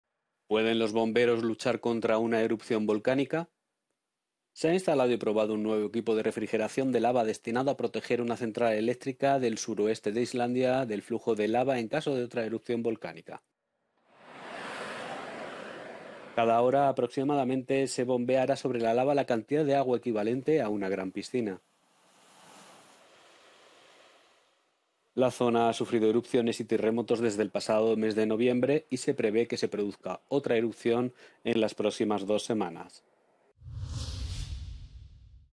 ¿Se puede desactivar un volcán? Así intentan evitar una nueva erupción en Islandia
Los bomberos trabajan a contrarreloj en Islandia intentando enfriar distintas zonas del volcán como plan para evitar una nueva erupción, o al menos, minimizar los daños.